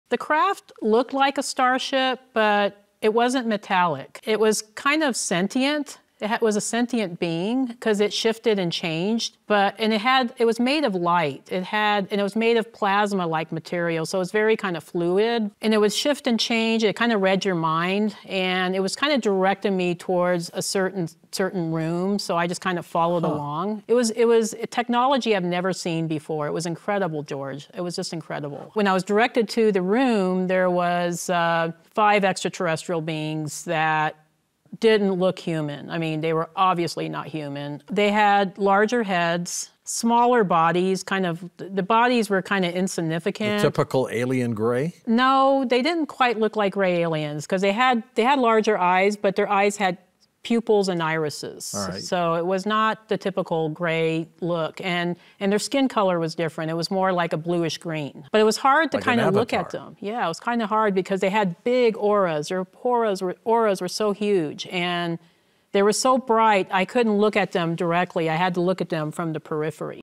Debbie Solaris - MY Story 2
🌟👽 Watch this mind-blowing video of a sentient, shape-shifting craft made of light and plasma leading to a room with non-human extraterrestrial beings! 😱👀